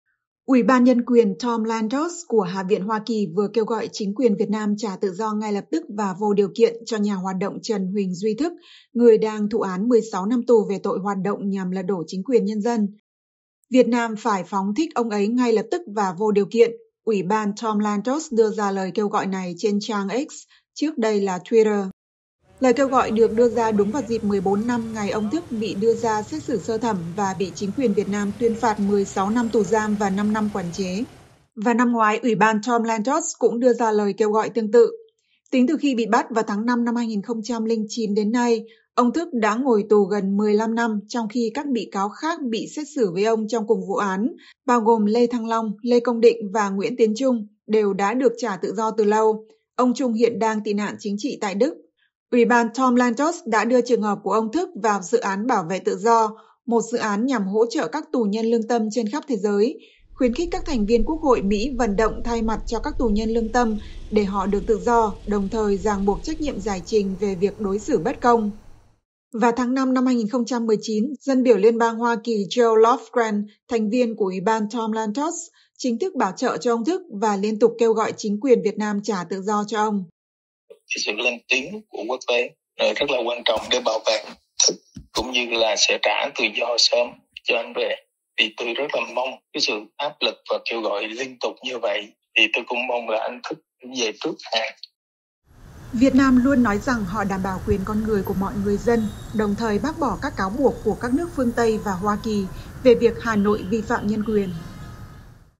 Ủy ban Nhân quyền Hạ viện Mỹ kêu gọi phóng thích Trần Huỳnh Duy Thức | VOA Tiếng Việt
Uỷ ban Nhân quyền Tom Lantos thuộc Hạ viện Hoa Kỳ vừa kêu gọi chính quyền Việt Nam trả tự do “ngay lập tức và vô điều kiện” cho nhà hoạt động Trần Huỳnh Duy Thức, người đang thụ án 16 năm tù về tội ‘Hoạt động nhằm lật đổ chính quyền nhân dân’.

“Việt Nam phải phóng thích ông ấy ngay lập tức và vô điều kiện”, Uỷ ban Tom Lantos đưa ra lời kêu gọi này trên trang X, trước đây là Twitter.

Lời kêu gọi được đưa ra đúng vào dịp 14 năm ngày ông Thức bị đưa ra xét xử sơ thẩm và bị chính quyền Việt Nam tuyên phạt ông 16 năm tù giam và 5 năm quản chế.

Vào năm ngoái, Ủy ban Tom Lantos cũng đưa ra lời kêu gọi tương tự.

Tính từ khi bị bắt vào tháng 5/2009 đến nay, ông Thức đã ngồi tù gần 15 năm trong khi các bị cáo khác bị xét xử với ông trong cùng vụ án, bao gồm Lê Thăng Long, Lê Công Định và Nguyễn Tiến Trung, đều đã được trả tự do từ lâu. Ông Trung hiện đang tị nạn chính trị tại Đức.

Uỷ ban Tom Lantos đã đưa trường hợp của ông Thức vào Dự án Bảo vệ Tự do (Defending Freedoms Project – DFP), một dự án nhằm hỗ trợ các tù nhân lương tâm trên khắp thế giới, khuyến khích các thành viên Quốc hội Mỹ vận động thay mặt cho các tù nhân lương tâm để họ được tự do, đồng thời ràng buộc trách nhiệm giải trình về việc đối xử bất công.

Vào tháng 5/2019, Dân biểu Liên bang Hoa Kỳ Zoe Lofgren, thành viên của Ủy ban Tom Lantos, chính thức bảo trợ cho ông Thức và liên tục kêu gọi chính quyền Việt Nam trả tự do cho ông.

Trần Huỳnh Duy Tân, em của Trần Huỳnh Duy Thức, nói: “Sự lên tiếng của quốc tế rất là quan trọng để bảo vệ anh Thức, cũng như là sẽ trả tự do sớm cho ảnh về. Tôi mong sự áp lực và kêu gọi liên tục như vậy sẽ giúp anh Thức được về trước hạn”.

Việt Nam luôn nói rằng họ “bảo đảm quyền con người của mọi người dân”, đồng thời các bỏ các cáo buộc của các nước phương tây và Hoa Kỳ về việc Hà Nội vi phạm nhân quyền.